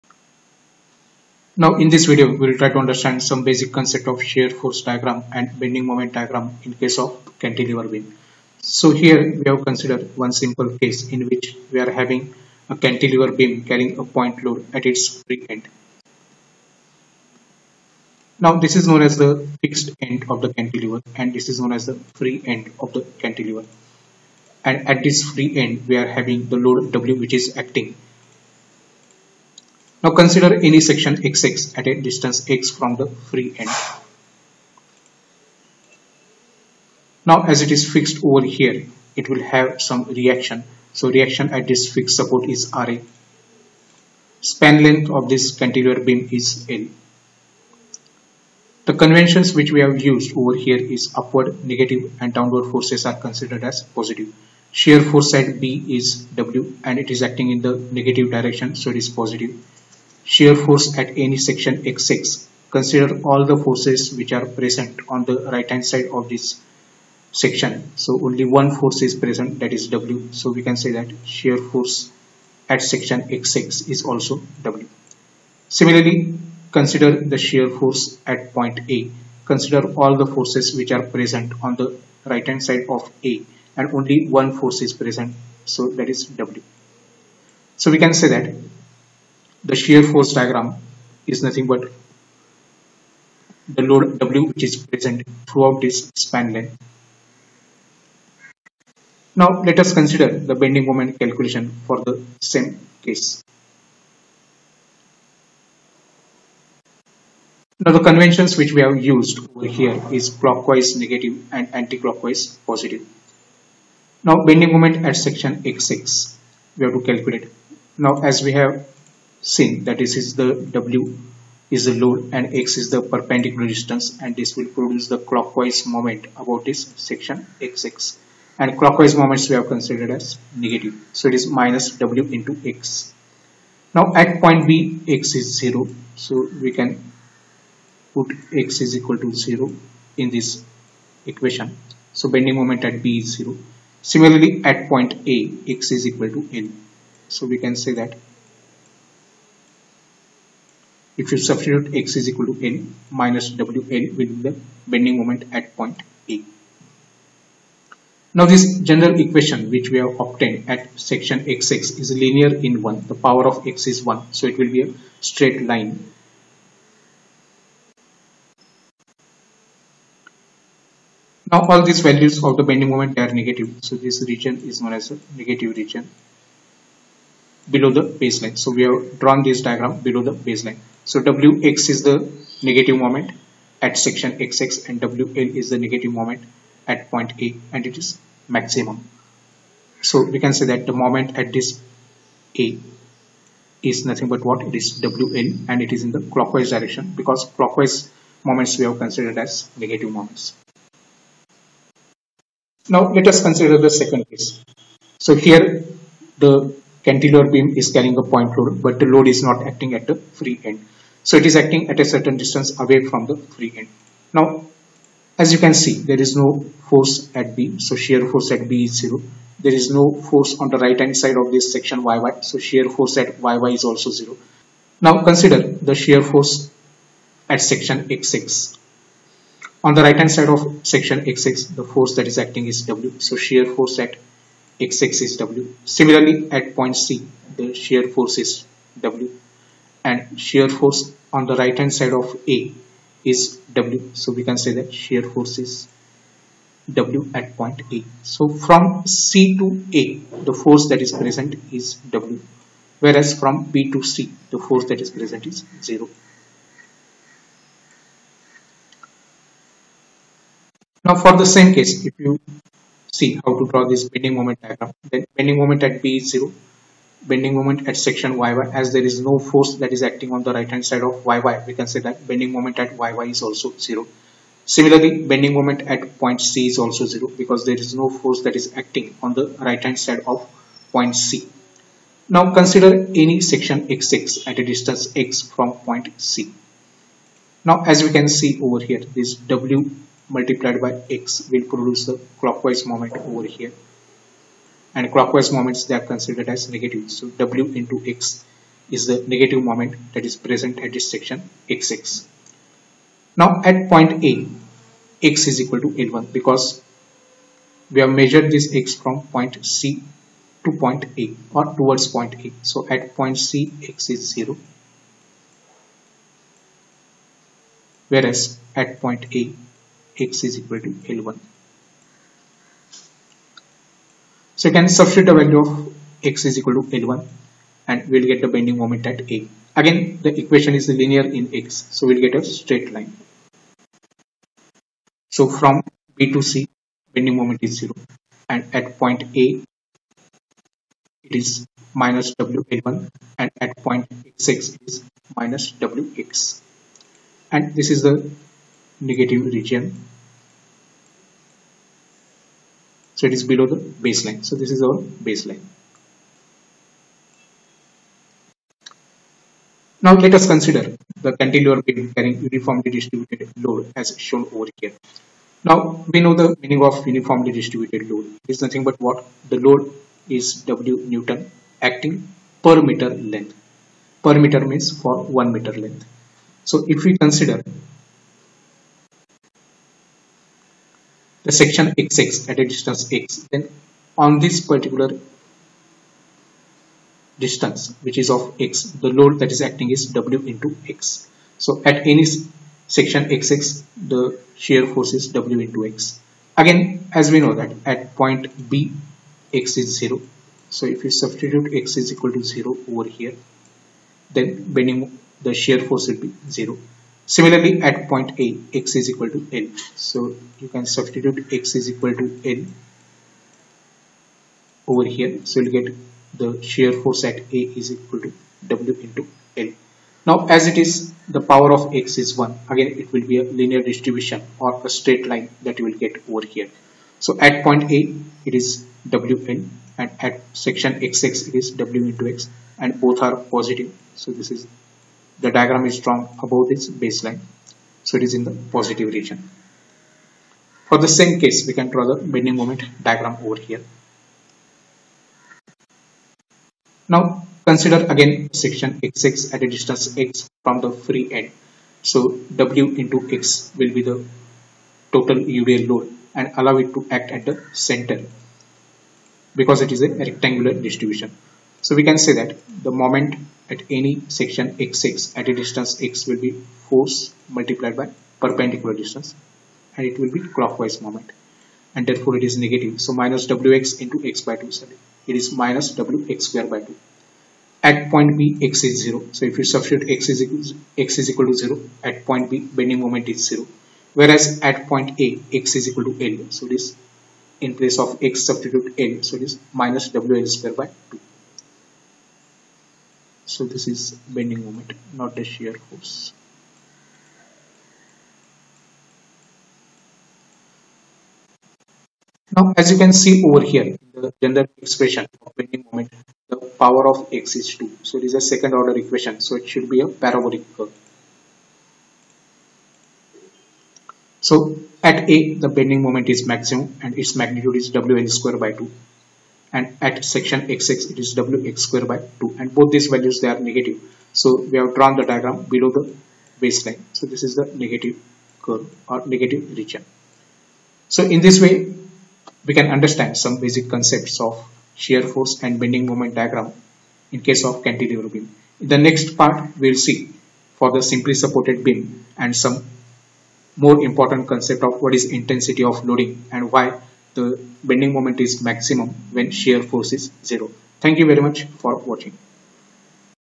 S.F.D. & B.M.D. basics PART 1
This video explains basic concepts of shear force & bending moment diagram (S.F.D. & B.M.D.) in case of cantilever beam useful for examination.